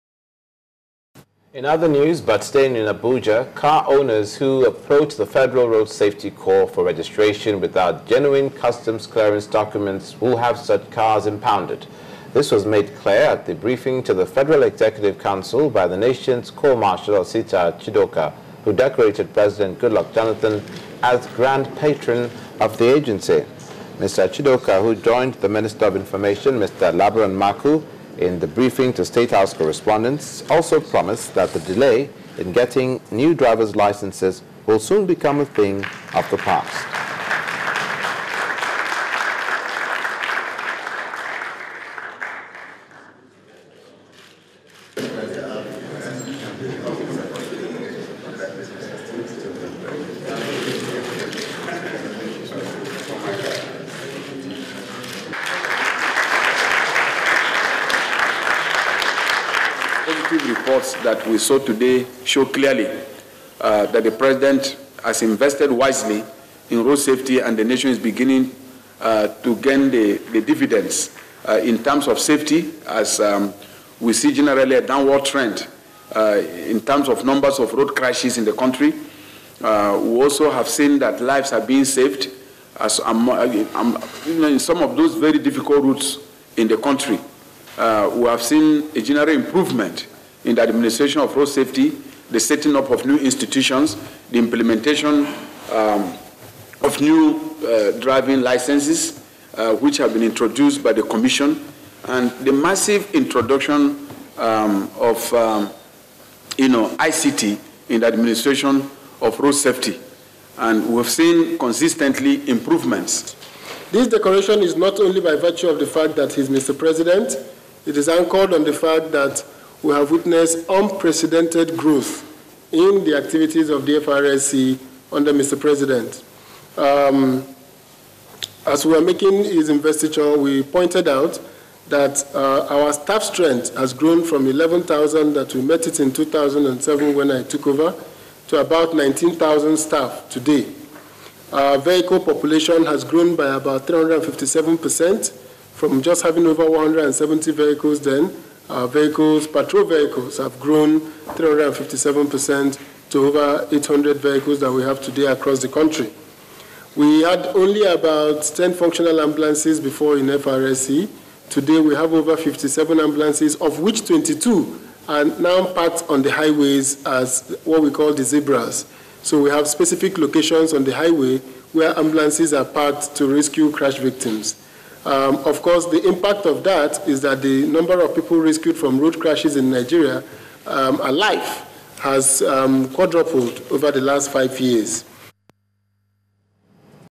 FEC Meeting: Jonathan Orders Seizure Of Cars With Fake Documents
President Jonathan directs FRSC to impound cars without genuine documents.